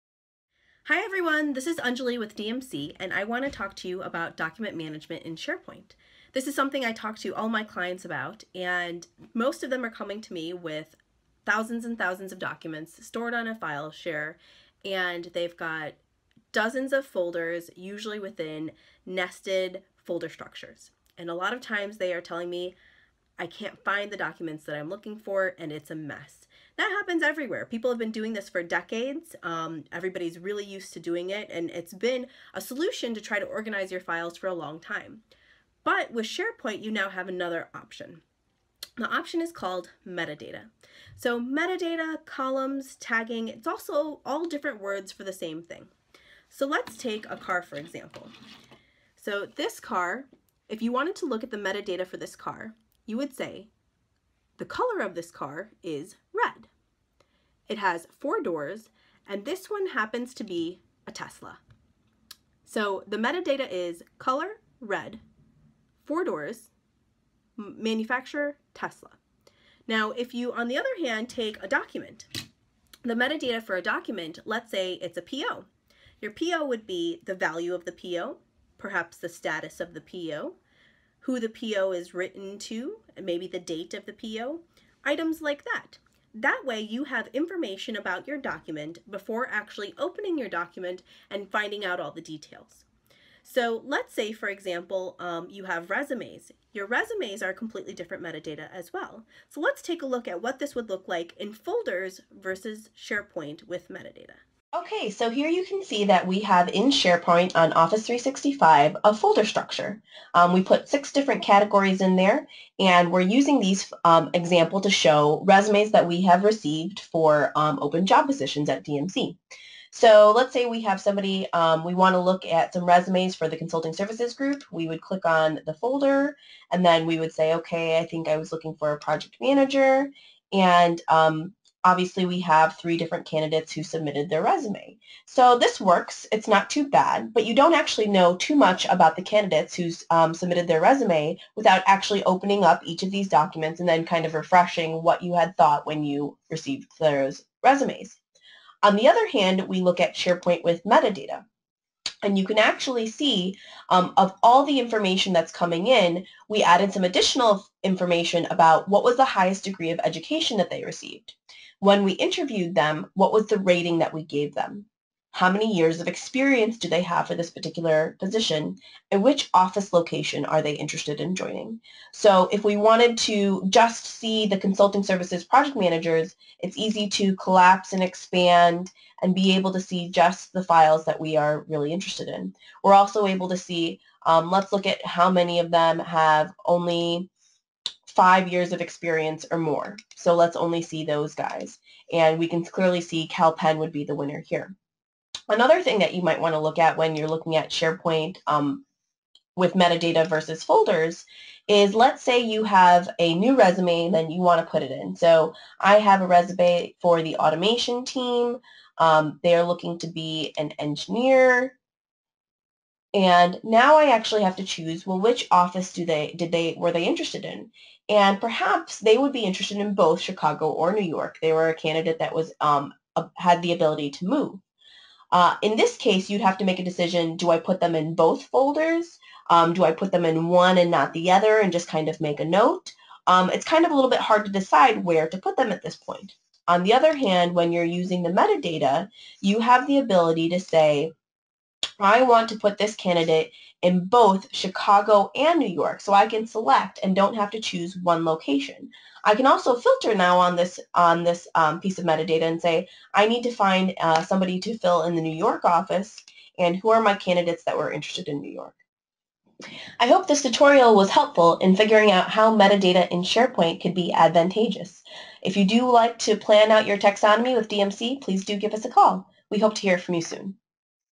How to Simplify Your Document Management with SharePoint Metadata
Have you ever had a hard time searching for a document in a web of nested folders? This quick video further explains how to simplify your document management with SharePoint metadata.